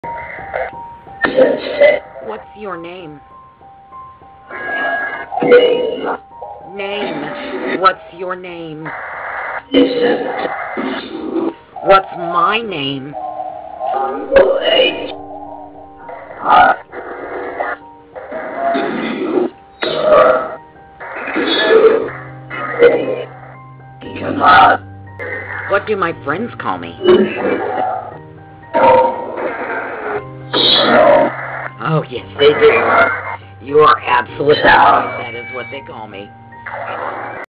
From The ITC Archives
Here is a short clip from my ITC archives. I have recorded many hours of communication with the other side. I hope to reconnect with this research in the near future.